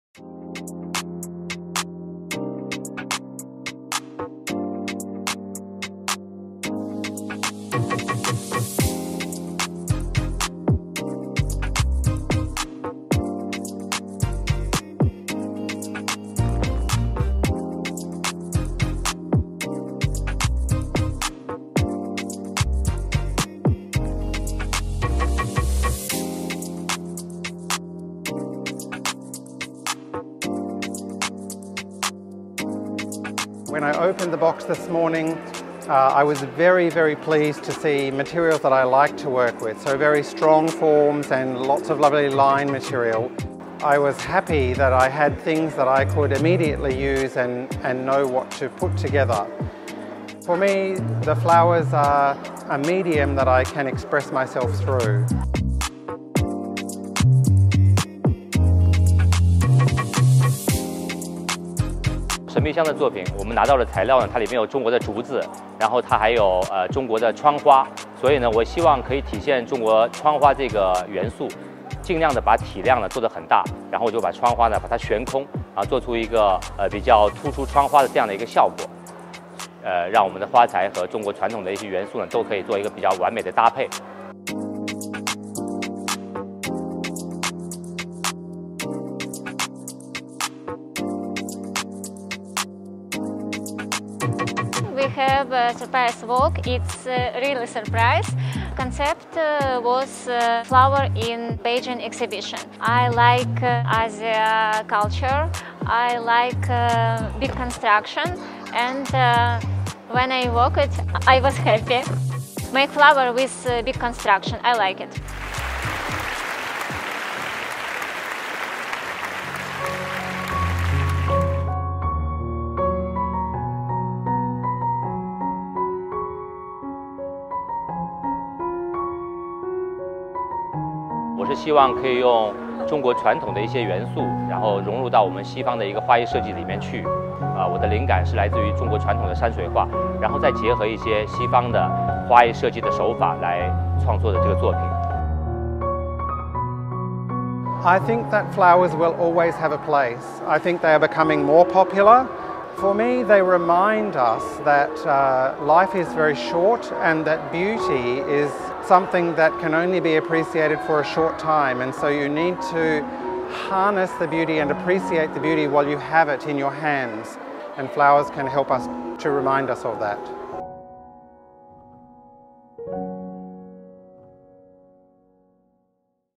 World Flower Art Contest: Top florists showcase the beauty of flower art
A total of 33 florists from 31 countries and regions competed in the 2019 World Flower Art Contest, held in Beijing from August 15 to 26, to showcase the beauty of flower art. The contest is the first international flower art competition held during the International Horticultural Exhibition.